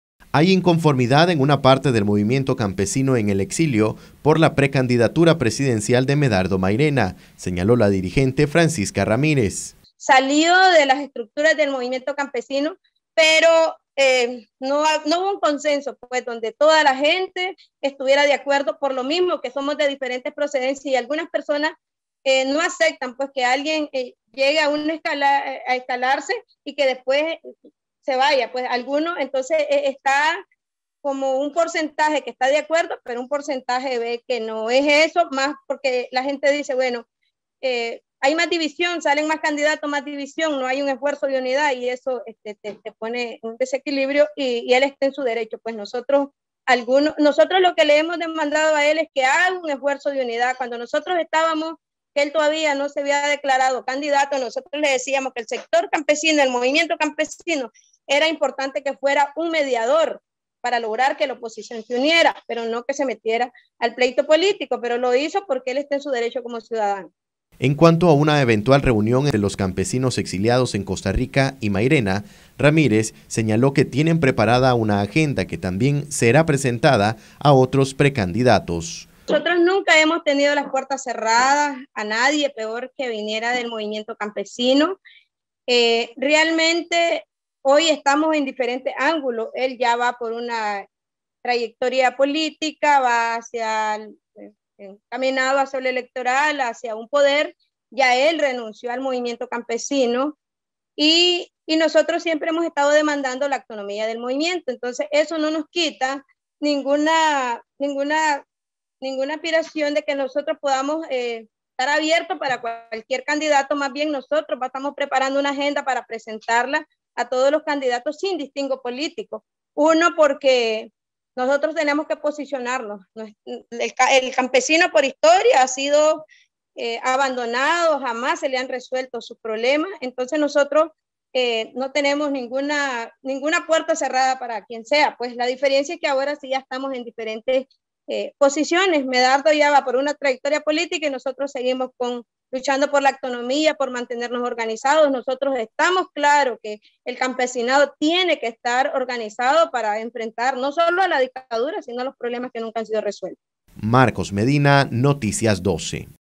Precandidatura de Medardo Mairena genera inconformidad en algunos sectores campesinos.
NosResistimosAlEmbargo Precandidatira de Medardo Mairena genera inconformidad en algunos sectores campesinos.